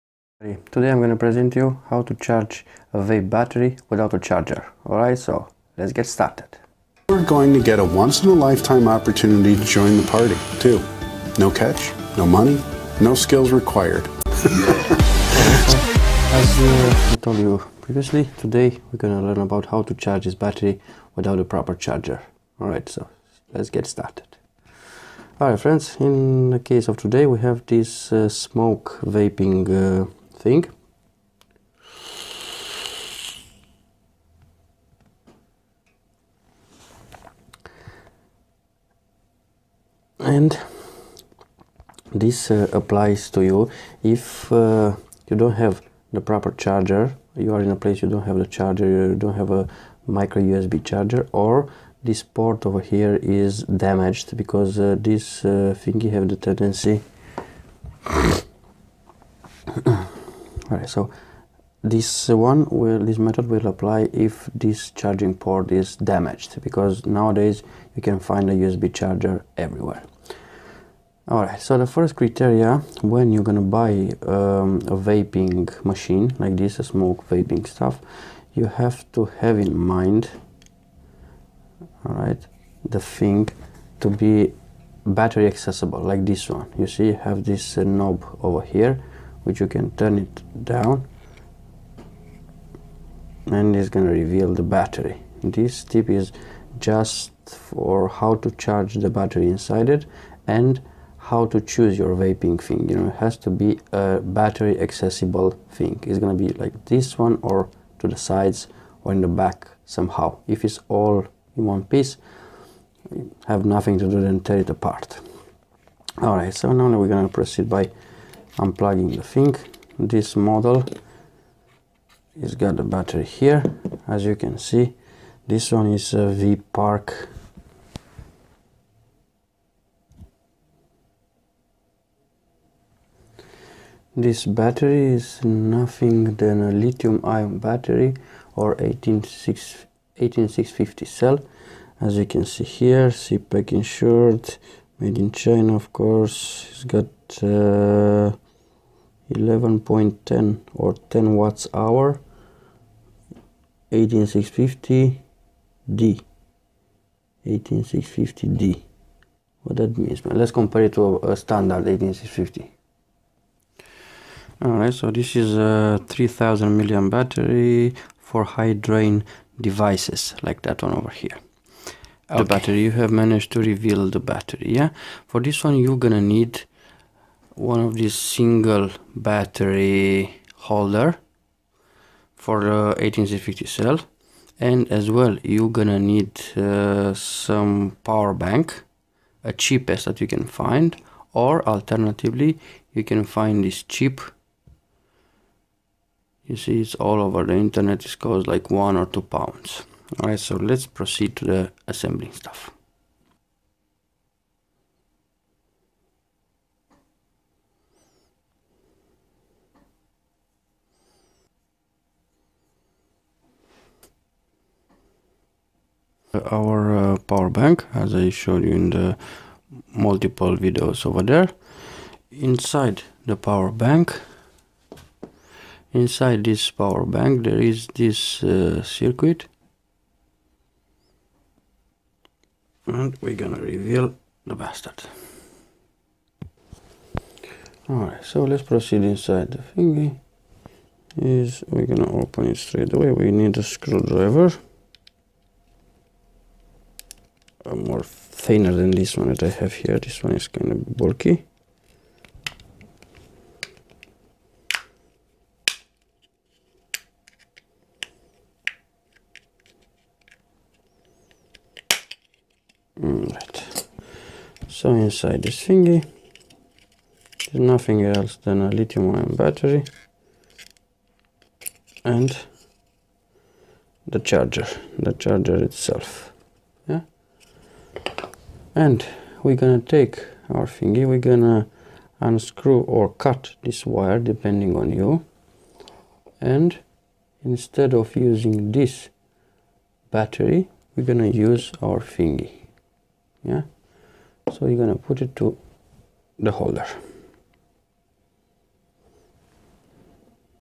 how to charge vape battery without a charger
Inside a vape machine or electronic cigarette, there is nothing else than a 18650 li-ion rechargeable battery that has more likely a similar chip to tp4056 for charging and discharging the lithium cell
.How to charge a vape battery cigarette without the charger is very simple if you have a battery accessible vape machine.If you have that just remove the battery from the vape machine and make a homemade li-ion battery charger with the help of a tp4056 or a cheap power bank this is no rocket science you can do it my friend.
Thanks one again  DIY Electronics rullez !!!